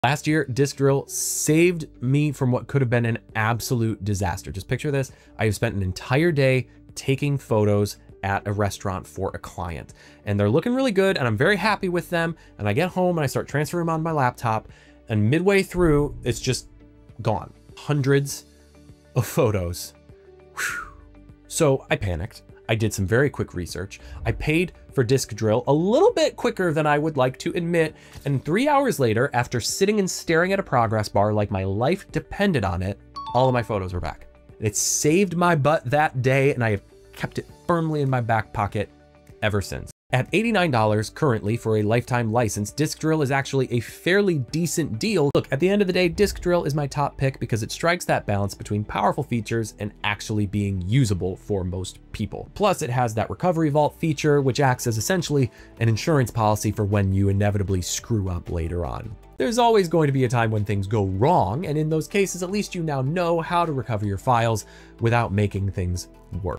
Best Data Recovery Software Saved My Client's Photos Disaster
Disk Drill rescued hundreds of client photos after a transfer failure! 🔍